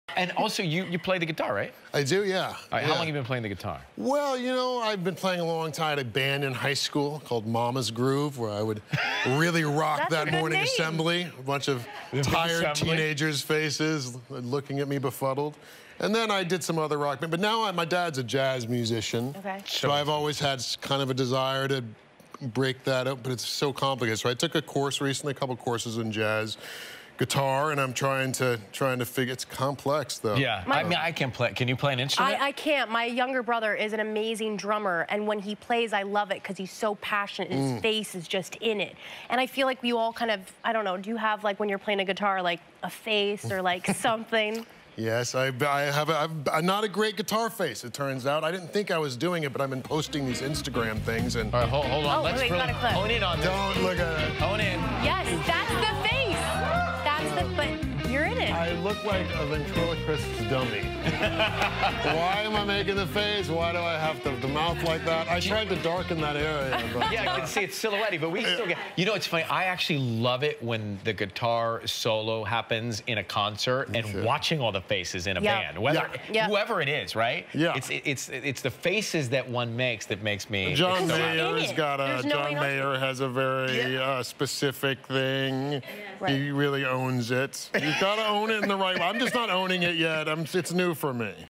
Jeremy Sisto's Guitar Face
Jeremy Sisto talks about playing guitar and working on the face he makes while playing.